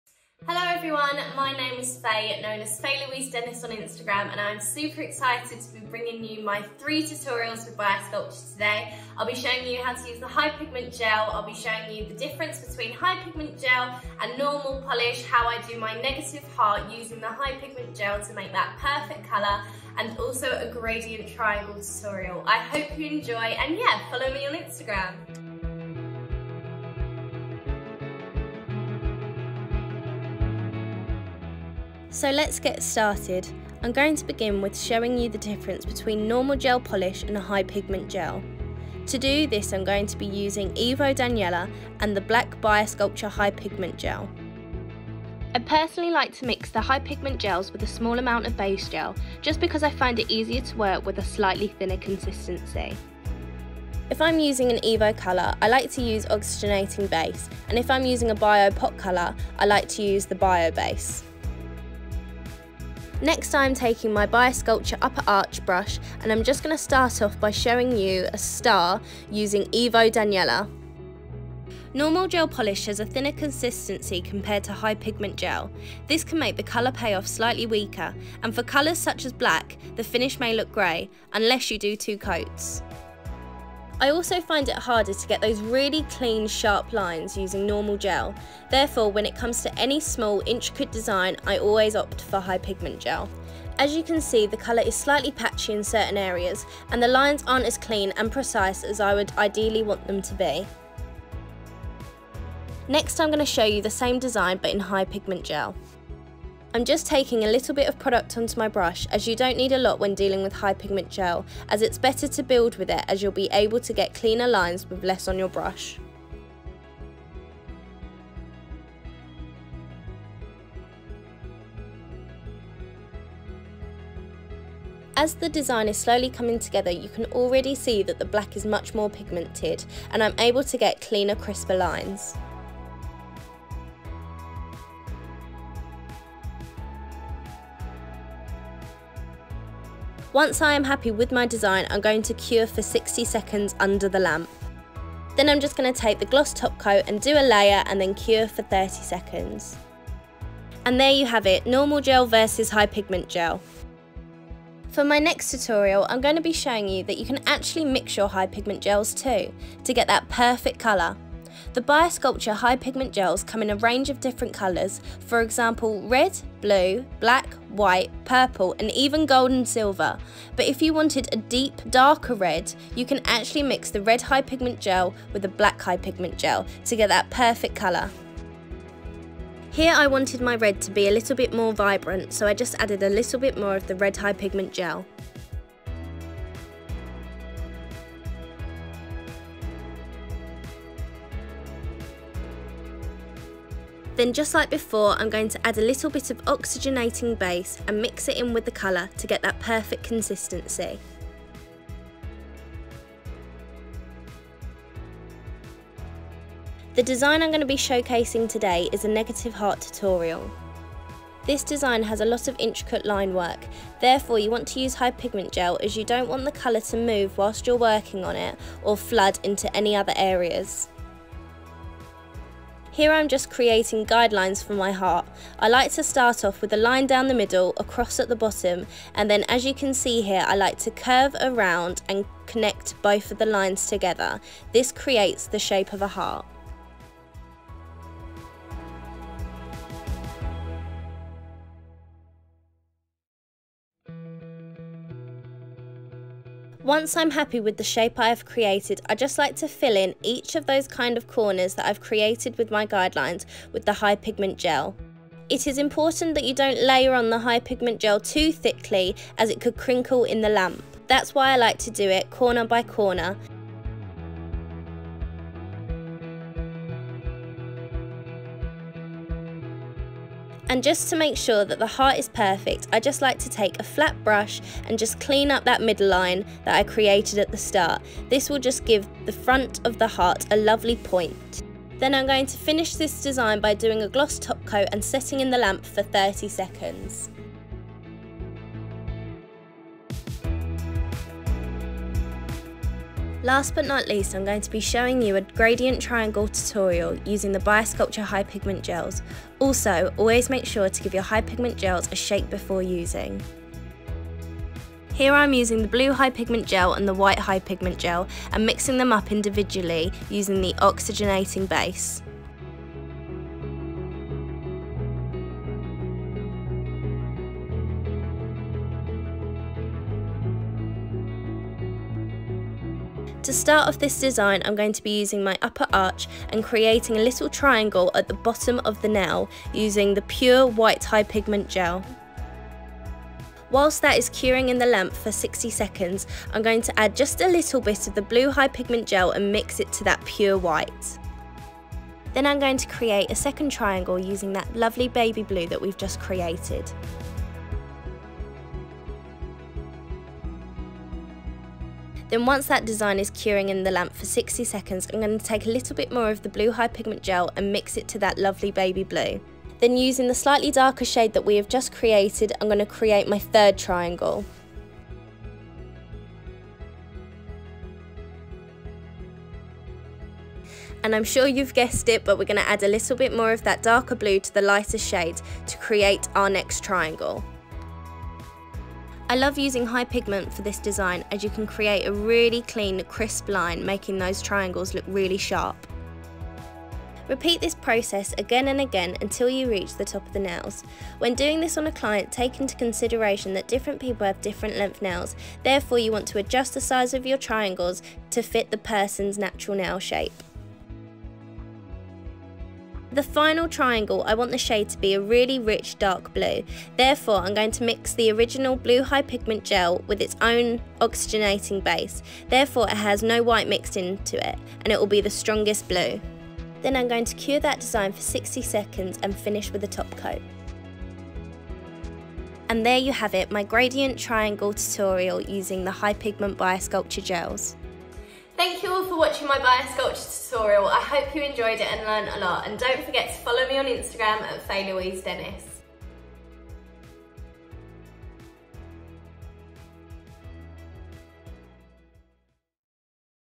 High Pigment Gels with Faye Dennis | Bio Sculpture Tutorial
Bio Sculpture mentor Faye Dennis is here to share her tips, tricks and designs using High Pigment Gels. Learn why these gels are perfect for bold, precise and colourful nail art!

Tutorial Contents:
0:00 - Intro
0:31 - HP Gel vs Gel polish
2:33 - Mixing your HP Gels
3:26 - Negative heart design
4:53 - Gradient triangle design
7:08 - Thank you for watching!

Find Bio Sculpture's High Pigment Gels in all 11 shades at renewbeauty.co.uk

Follow Faye Dennis at FayeLouiseDennis
Follow us at BioSculptureGelGB